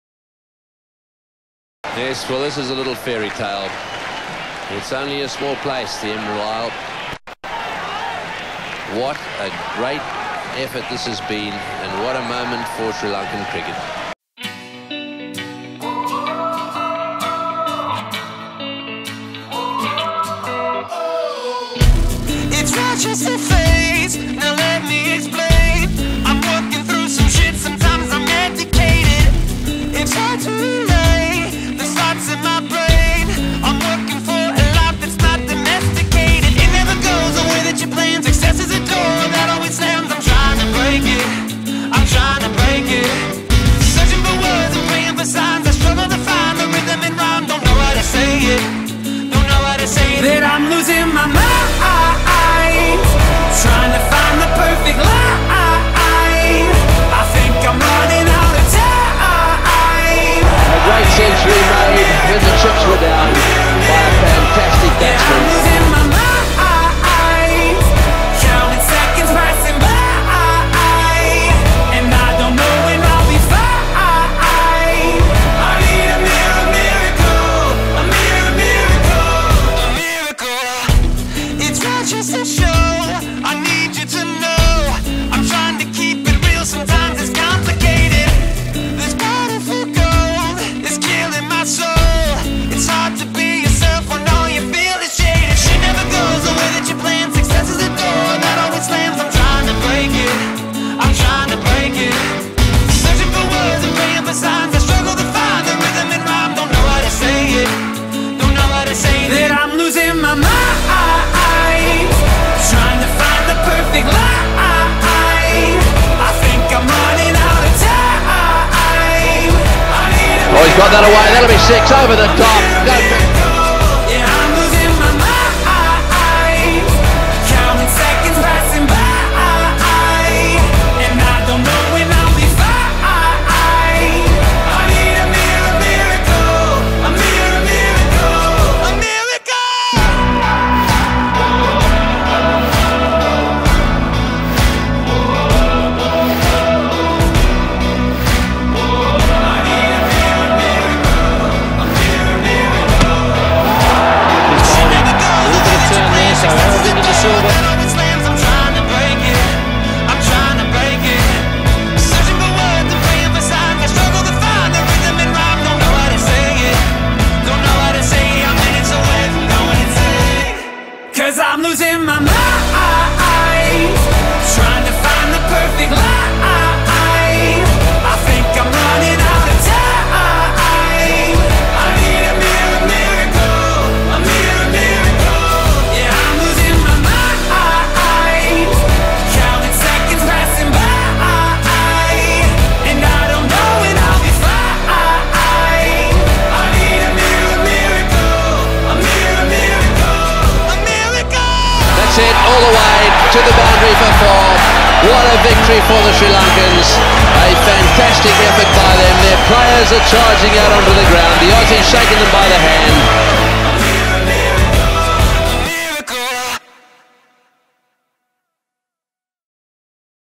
GOLDEN 90's | Sri Lanka Cricket Golden Era
24 Years ago 17th of March Sri Lanka Crowned as the Cricket champions in the world.

Sorry for the low quality video :(
Catch me on - Facebook // Instagram @chamika99 @graphixdc